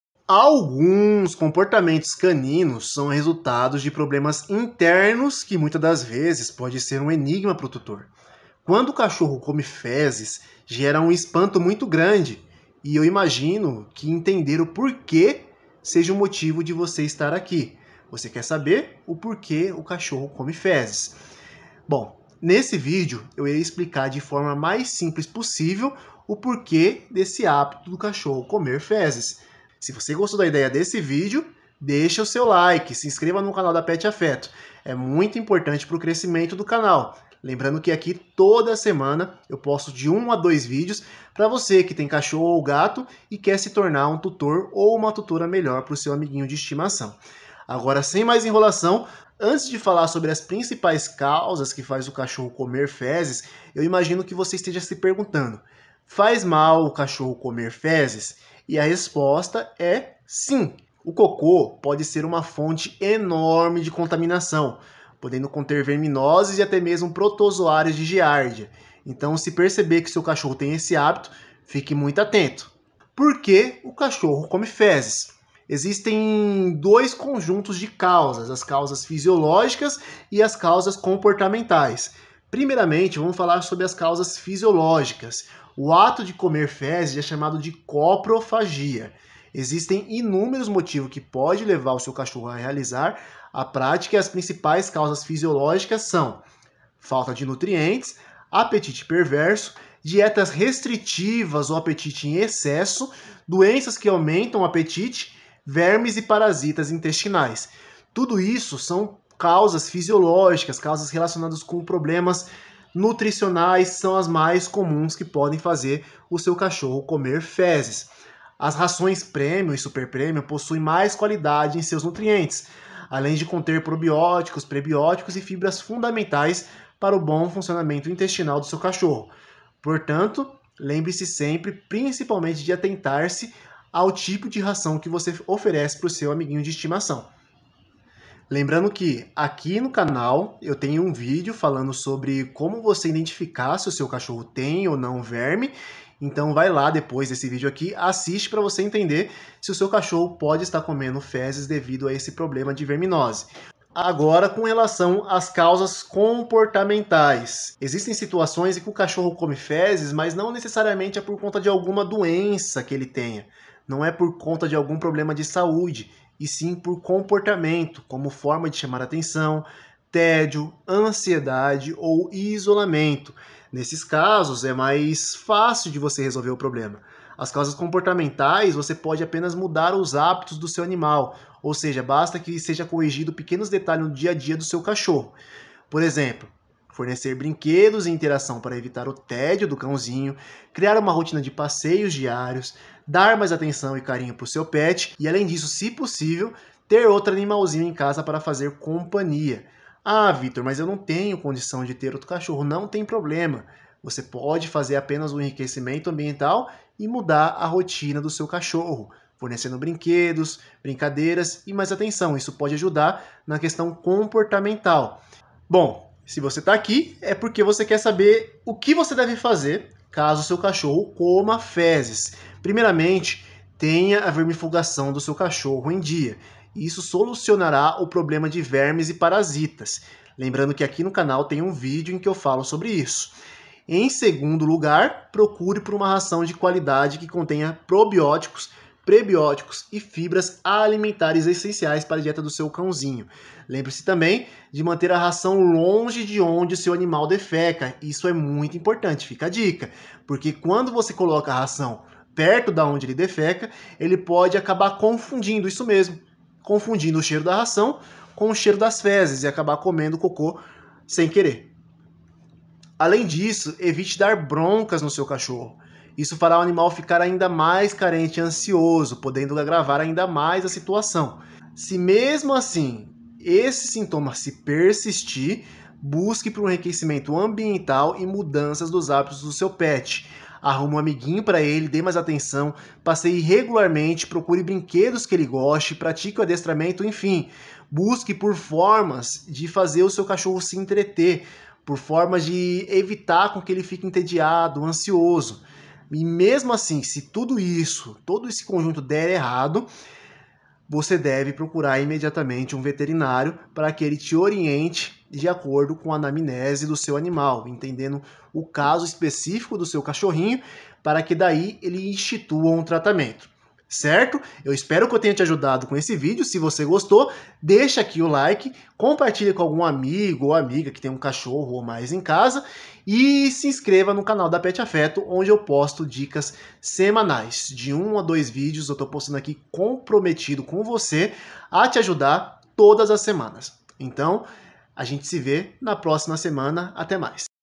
Meu cachorro come fezes! O que fazer?
Por que cachorro come fezes?
Bom muita gente tem essa dúvida, saiba que pode ser desde causas nutricionais ou até mesmo causas comportamentais.

Um cachorro comendo fezes definitivamente não é algo saudável e indica que algo está muito errado.

Nós da Pet afeto preparamos um esse vídeo para te ajudar a compreender porque cão come fezes. Aqui você irá entender também porque cachorro filhote come fezes.

Desta forma entenderá o que fazer caso seu cãozinho esteja praticando esse hábito. E entenderá de uma vez por todas como curar cachorro que come cocô.